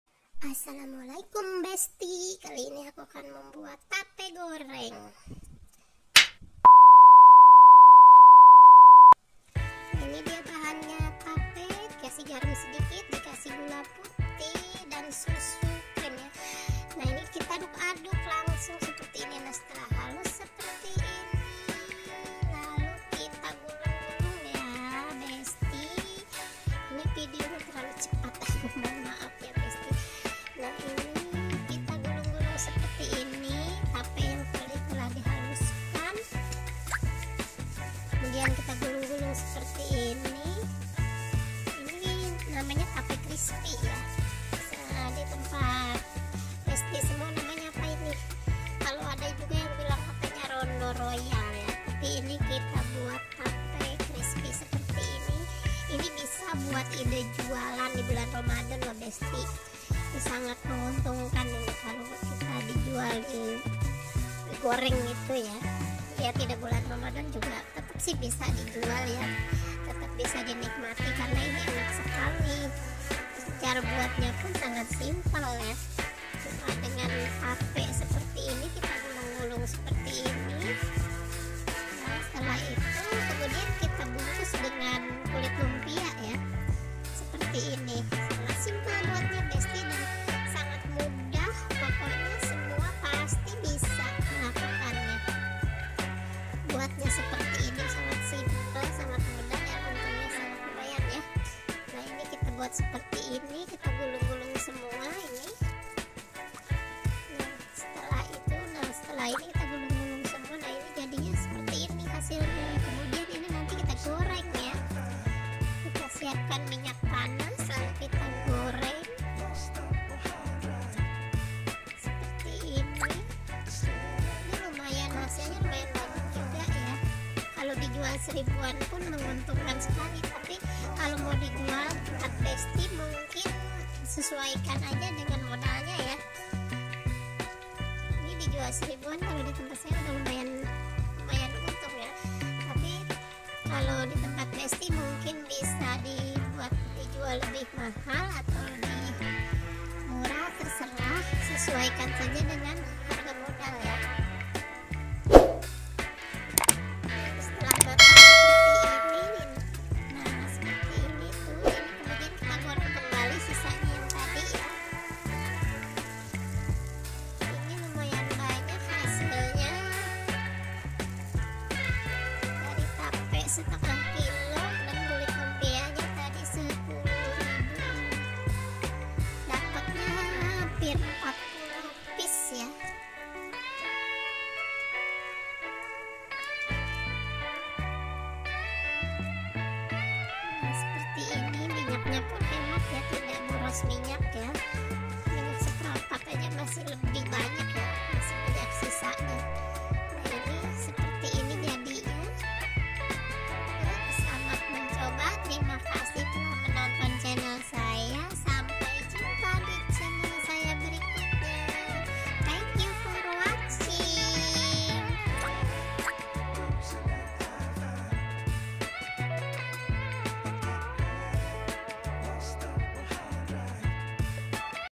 RONDO ROYAL // TAPE CRISPY IDE JUALAN KEKINIAN
assalamualaikum pemirsah kali ini saya akan berbagi resep membuat Rondo royal atau yang lebih keren disebut tape crispy
cara buat nya pun mudah bahannya pun simpel
 1/2 Kg tape
50 lembar kulit lumpia
100g gula putih
minyak goreng
garam sedikit